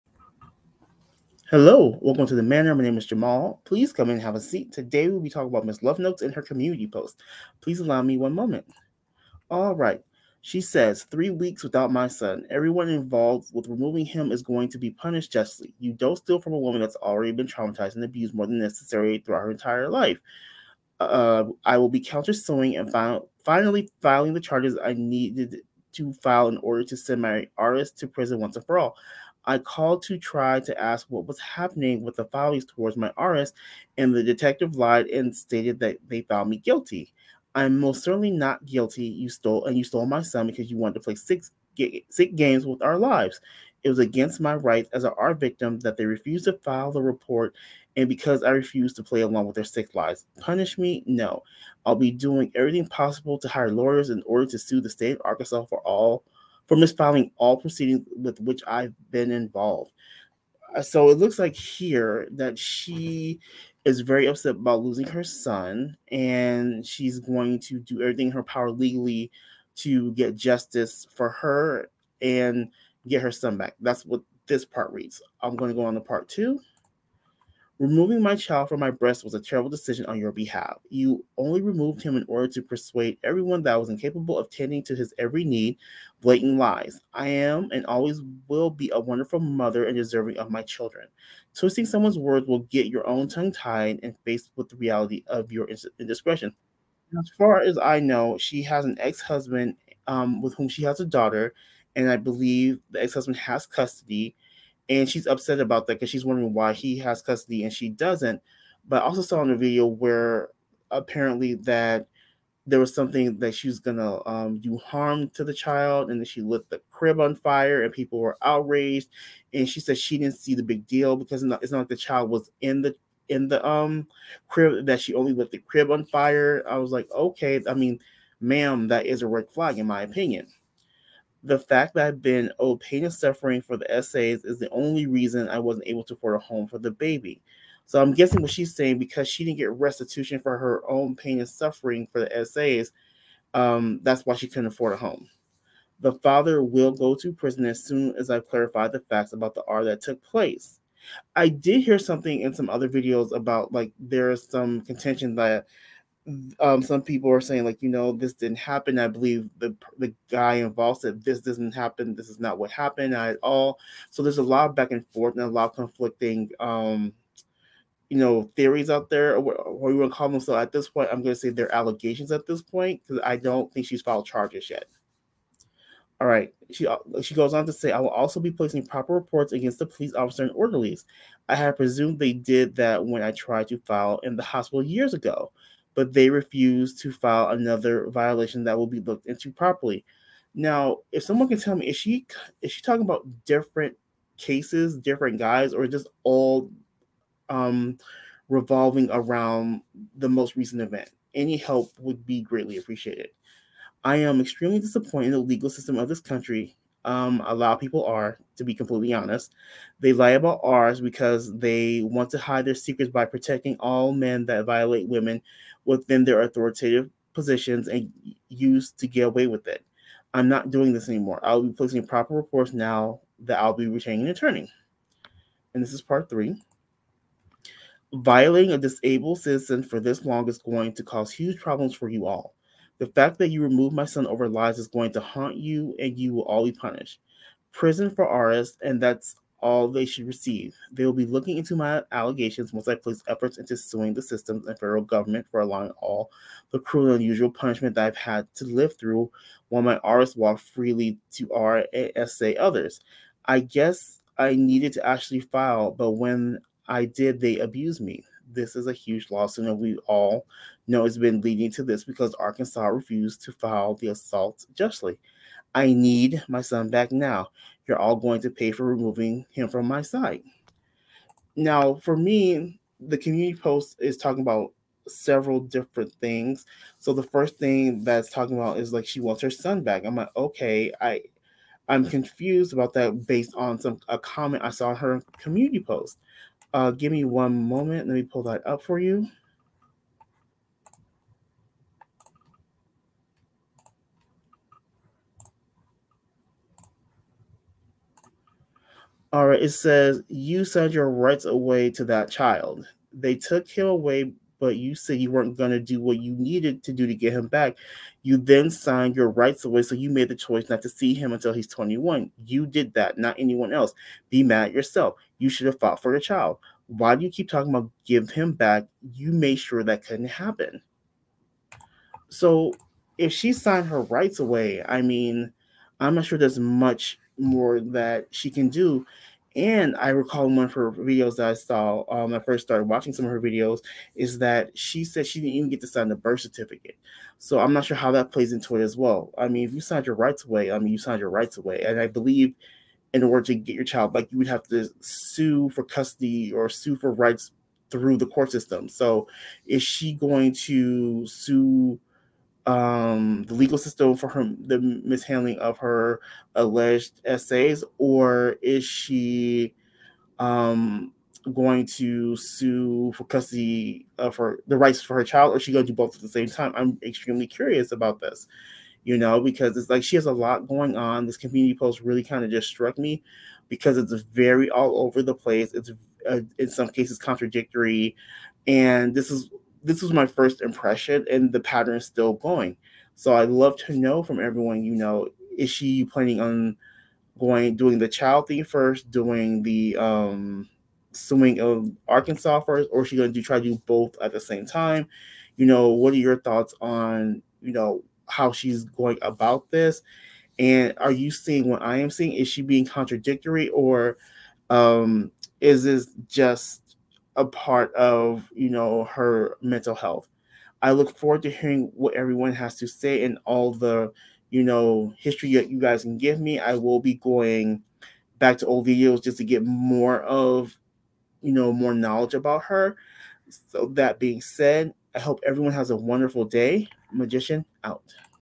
Love Notes For TLC: Lawsuit?
It appears that Leticia will be suing some people. Does she have a chance to win?